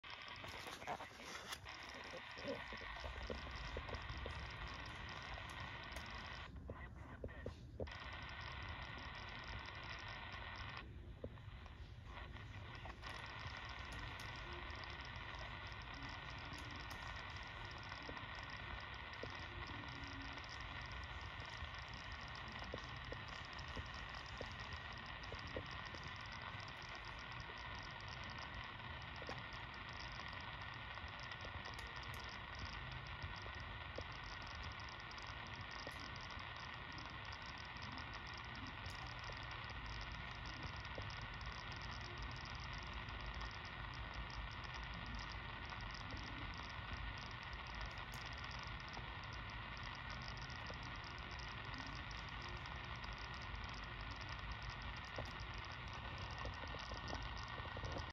36.450 MHz FM Russian (?) Comms 36.4500 FM Via G8JNJ Wessex UK SDR - February 6th 2023
Via G8JNJ SDR.  06-FEB-2023.  This is not my receiver.  Received via remote Internet receiver in the UK.